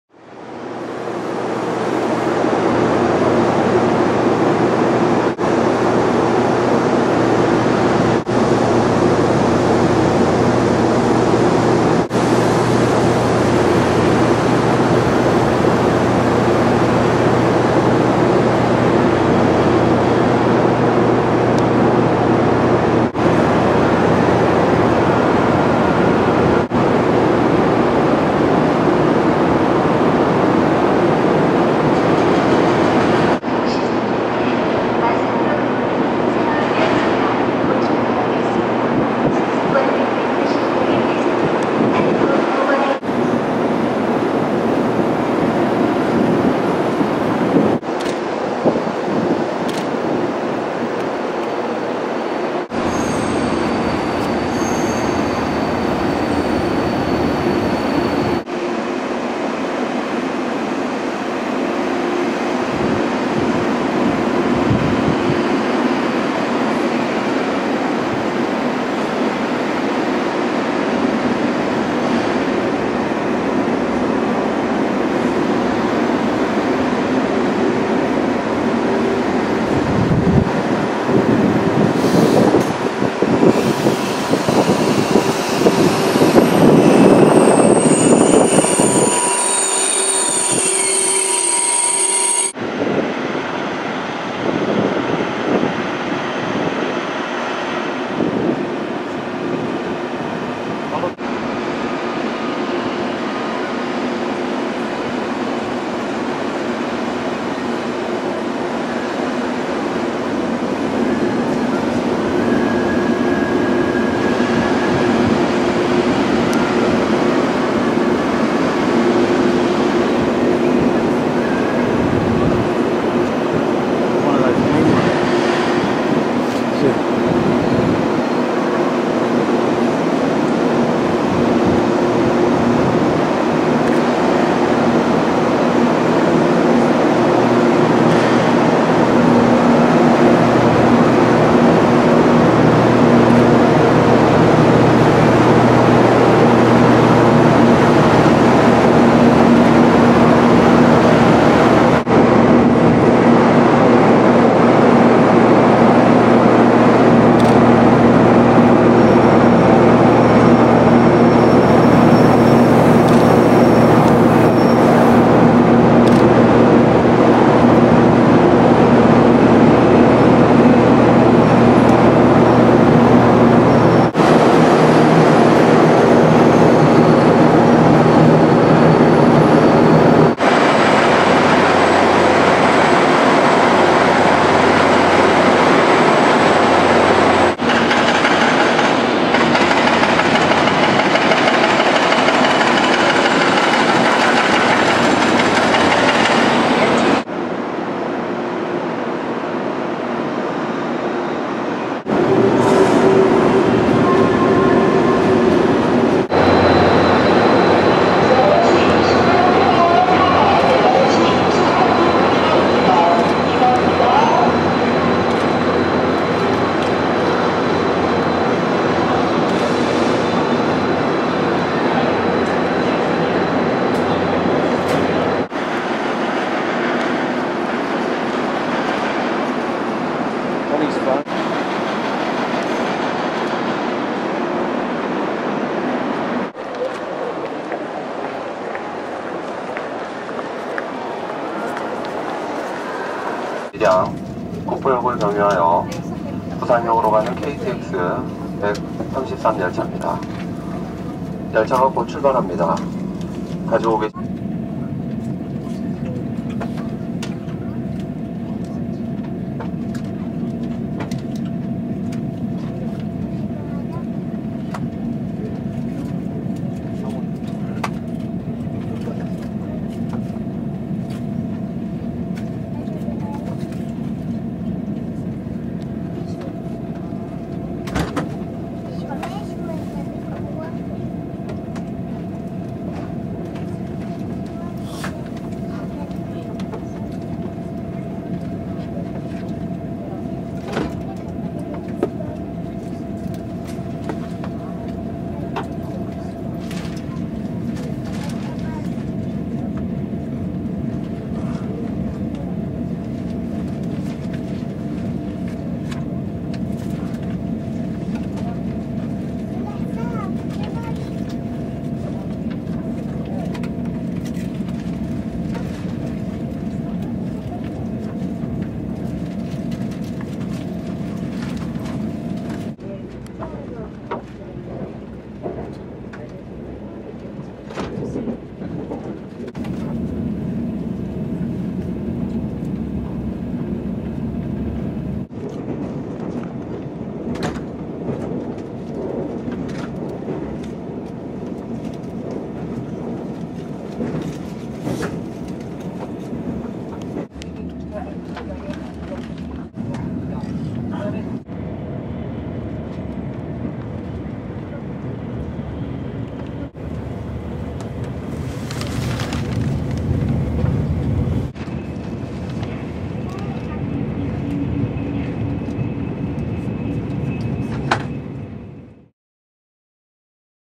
Korean trains- KTX From Seoul to Busan
2010, taking the KTX high speed train from Seoul South Korea to the southern port city of Busan.